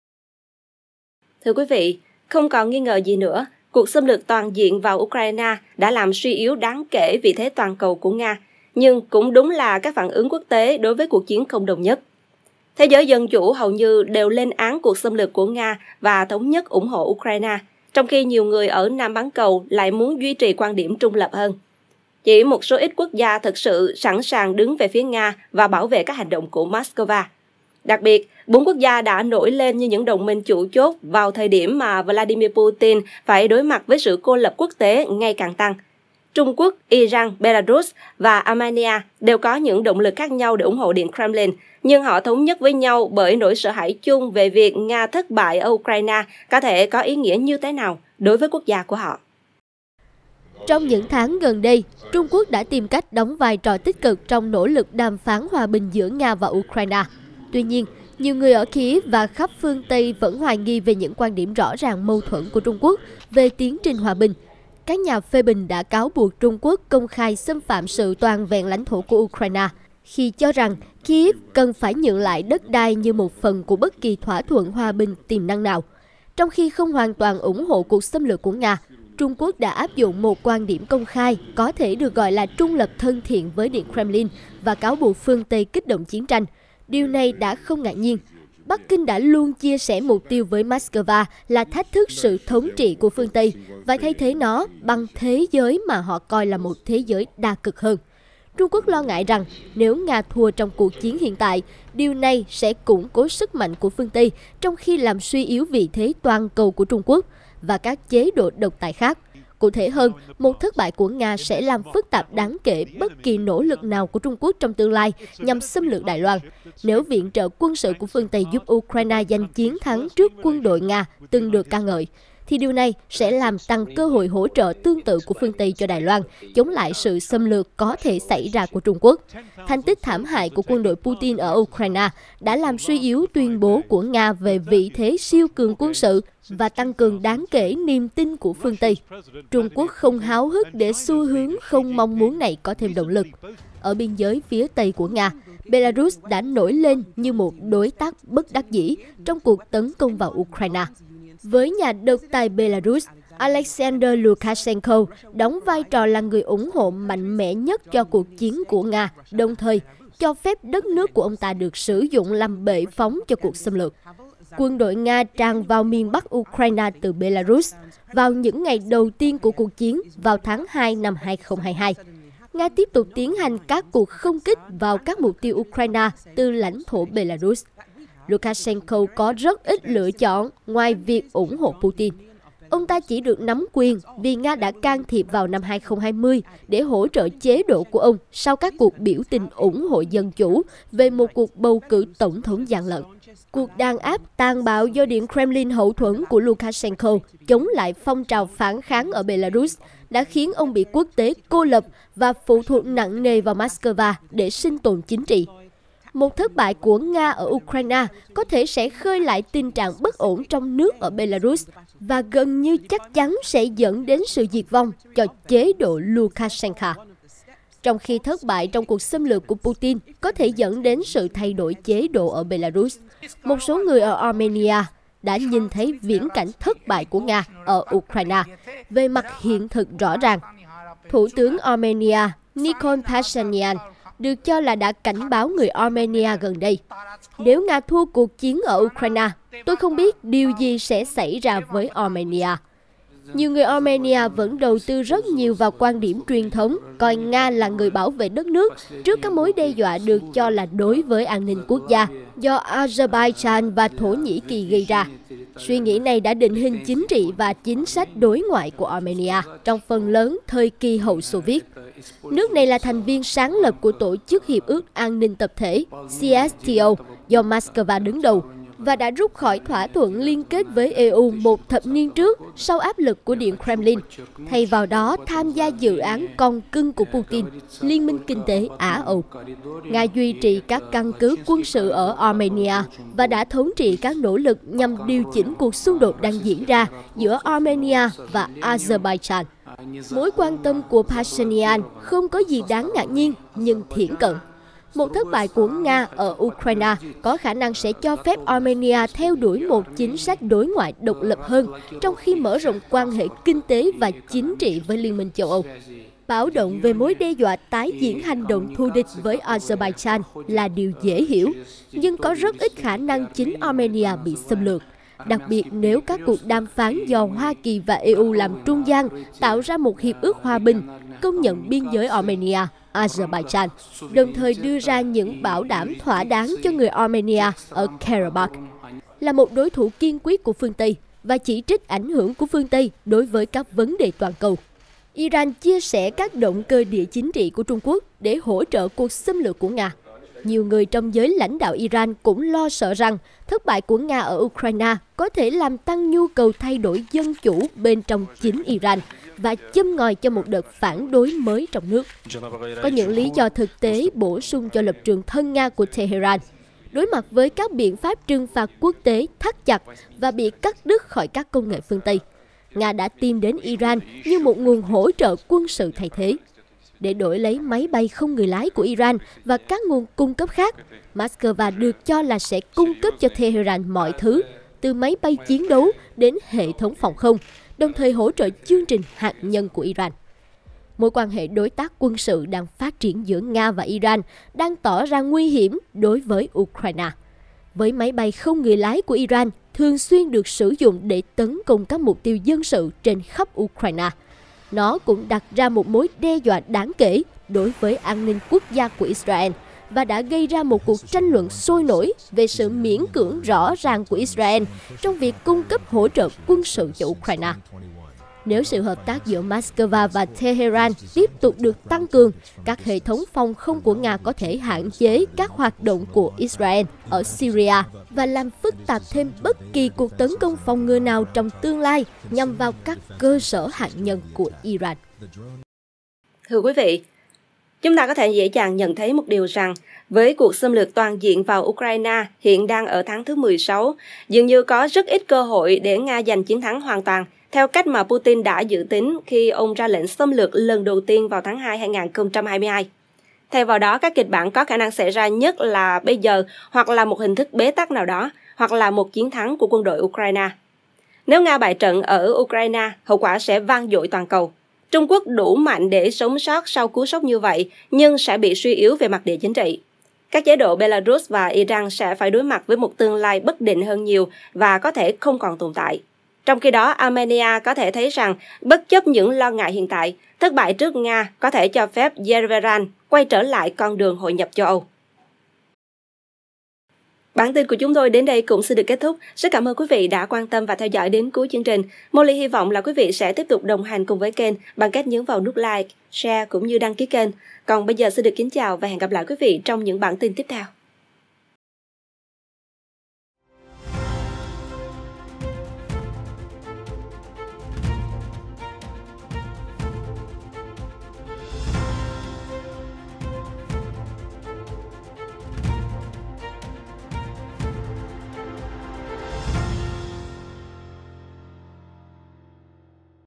“Sự đồng tình bất đắc dĩ” - Nỗi khổ khó nói của fan cứng điện Kremlin
Không còn nghi ngờ gì nữa, cuộc xâm lược toàn diện vào Ukraine đã làm suy yếu đáng kể vị thế toàn cầu của Nga, nhưng cũng đúng là các phản ứng quốc tế đối với cuộc chiến không đồng nhất. Thế giới dân chủ hầu như đều lên án cuộc xâm lược của Nga và thống nhất ủng hộ Ukraine, trong khi nhiều người ở Nam bán cầu muốn duy trì quan điểm trung lập hơn.
Chỉ một số ít quốc gia thực sự sẵn sàng đứng về phía Nga và bảo vệ các hành động của Moscow. Đặc biệt, bốn quốc gia đã nổi lên như những đồng minh chủ chốt vào thời điểm mà Vladimir Putin phải đối mặt với sự cô lập quốc tế ngày càng gia tăng. Trung Quốc, Iran, Belarus và Armenia đều có những động lực khác nhau để ủng hộ Điện Kremlin, nhưng họ thống nhất với nhau bởi nỗi sợ hãi chung về việc Nga thất bại ở Ukraine có thể có ý nghĩa như thế nào đối với quốc gia của họ.
The Saigon Post - Tin tức Cập nhật, Nhân bản!